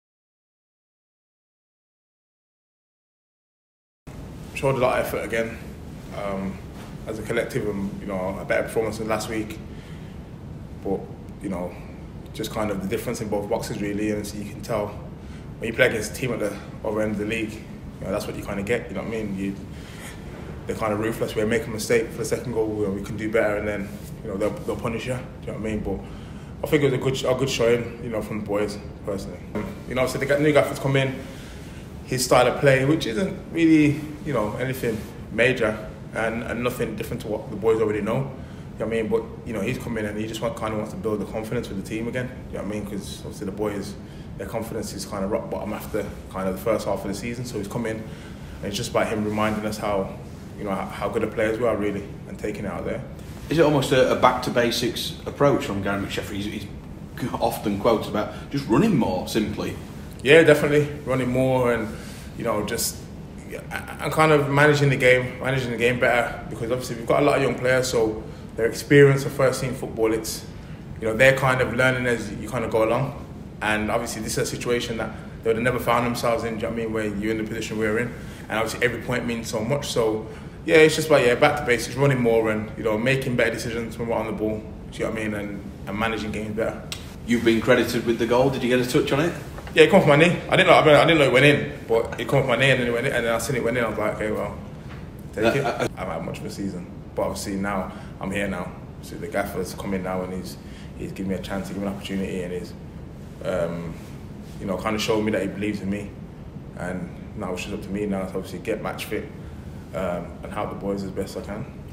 Omar Bogle discusses his goal and a much better performance
Omar Bogle scored with his first touch of the game after being introduced in the 60th minute as Rovers showed fight and spirit in defeat to Wigan Athletic.